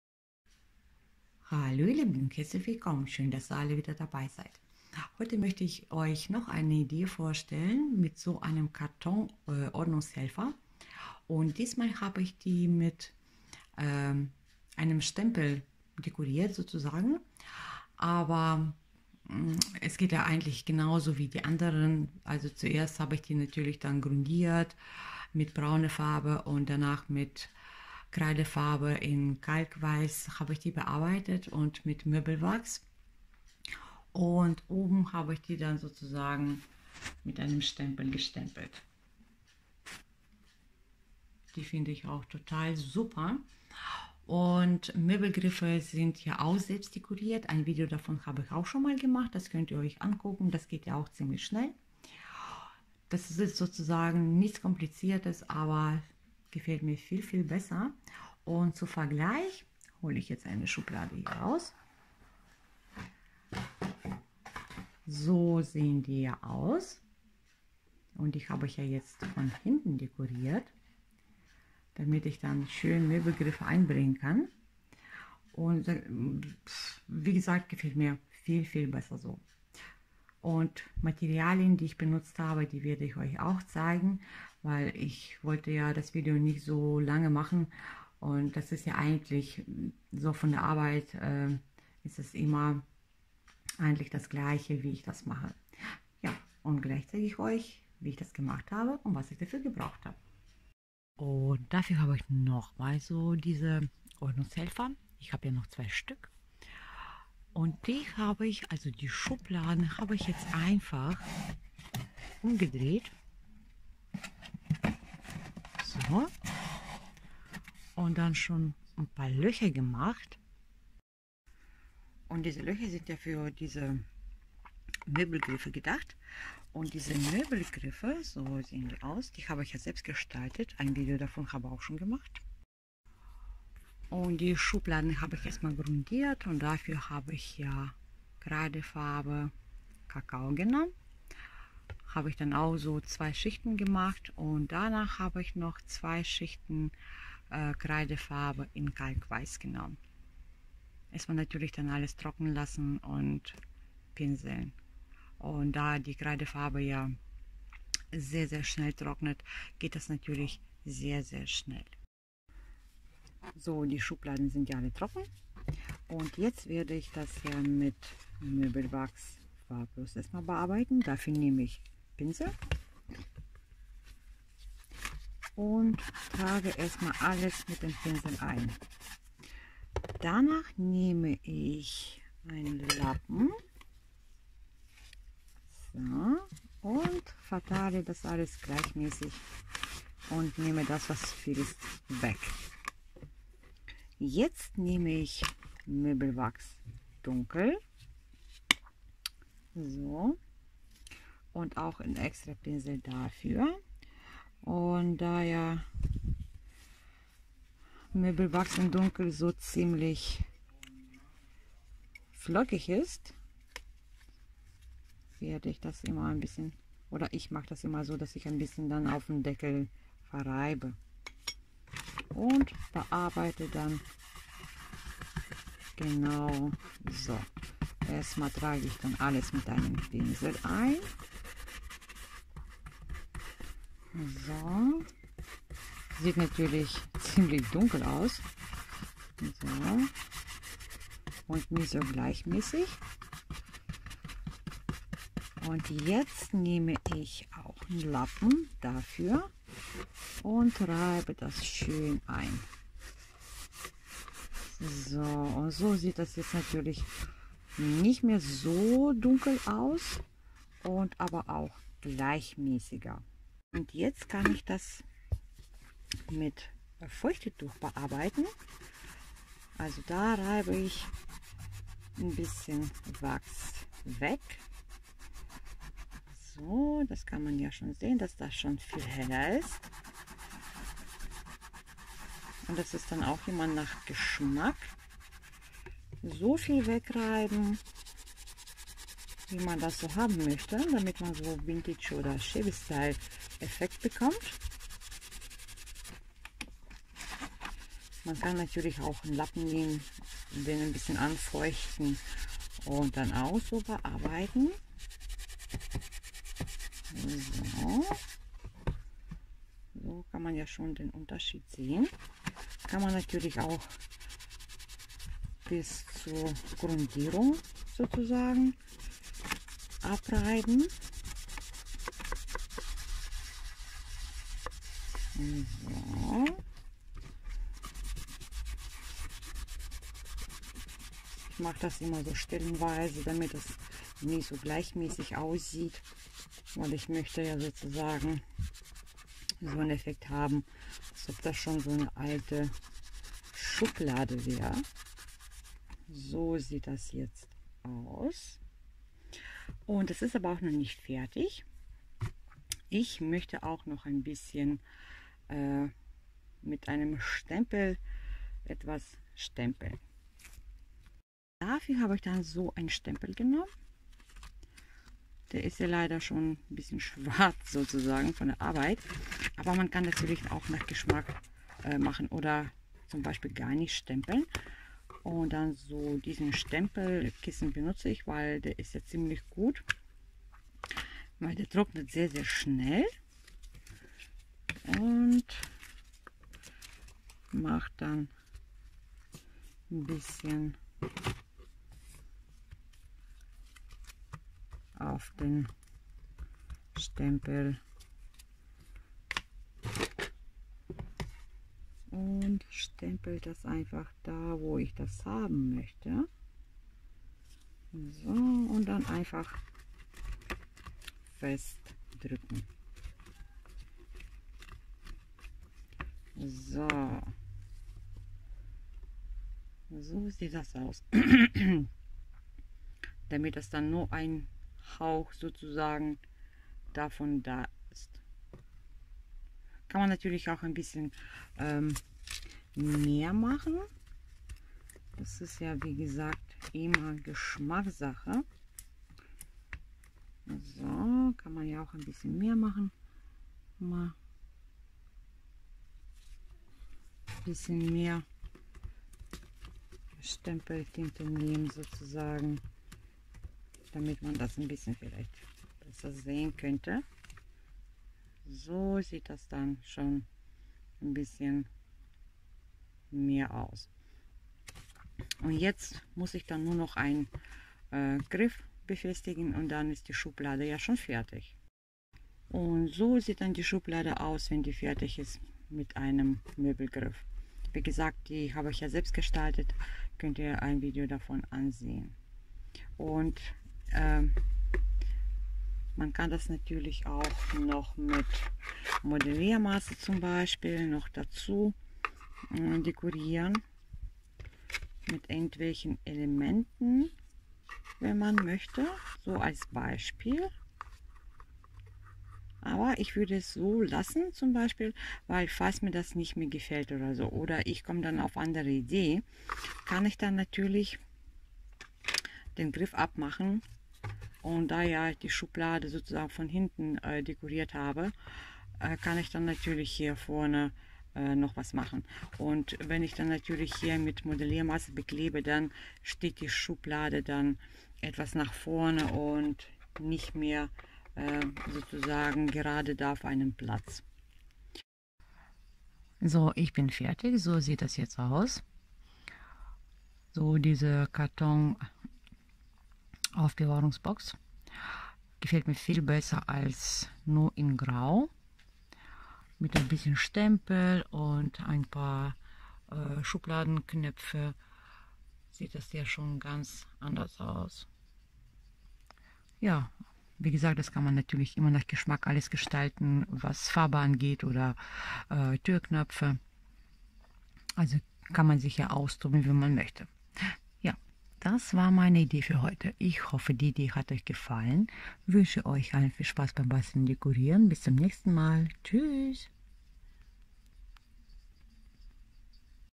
Ordnungshelfer umstyling Variante Nr. 2
Ich habe noch eine umstyling Variante für Ordnungshelfer gemacht.
Diesmalhabe ich die Schubladen mit einem Motivstempel und Möbelgriffen dekoriert. Sonst wie immer erstmal die Schubladen grundiert(in  braun) danach in weiß gestrichen und mit Möbelwachs bearbeitet.
Sonst ist es wie immer Gestaltung nach Geschmack.
Die Scubladen kann man mit Servietten bekleben, schablonieren, mit Ornamenten aus Holz oder Modelliermasse dekorieren usw.
Ich wünsche euch allen viel Spaß beim basteln und dekorieren.
Liebe Grüße
Elena
Materialien (alles von mir selbst gekauft)
*Ordnungshelfer mit Schubladen aus Karton
*Kreidefarbe Möbellack in Kakao und Kalkweiß von Rust-Oleum(Baumarkt)
*Möbelwachs farblos und dunkel von Rust-Oleum(Baumarkt)
*Möbelgriffe(Tedi oder Kik)
*Baby Feuchttücher
*Pinsel,Lappen
*Schrift Stempel (Amazon,Bastelladen)